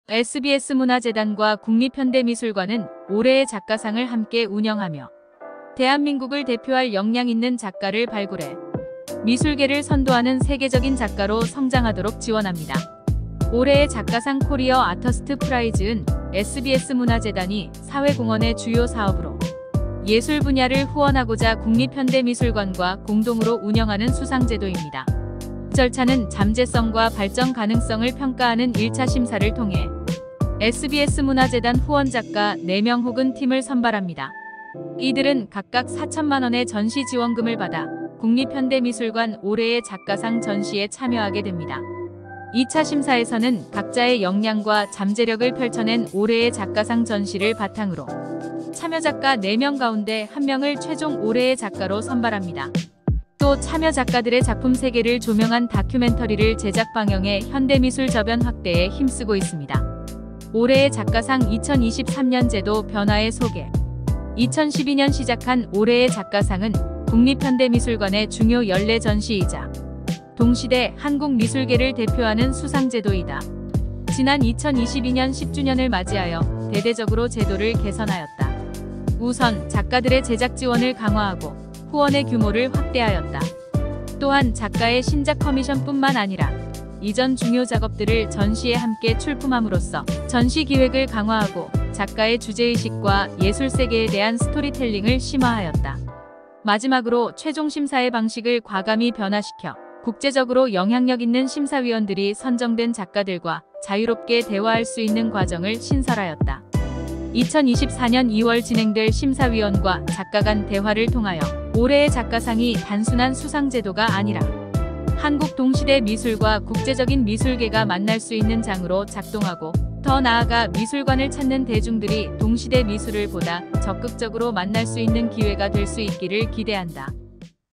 미술이벤트 / 올해의 작가상 ( Korea Artist Prize )이란?
올해의 작가상 Korea Artist Prize은 SBS문화재단이 사회공헌의 주요 사업으로 예술분야를 후원하고자 국립현대미술관과 공동으로 운영하는 수상제도..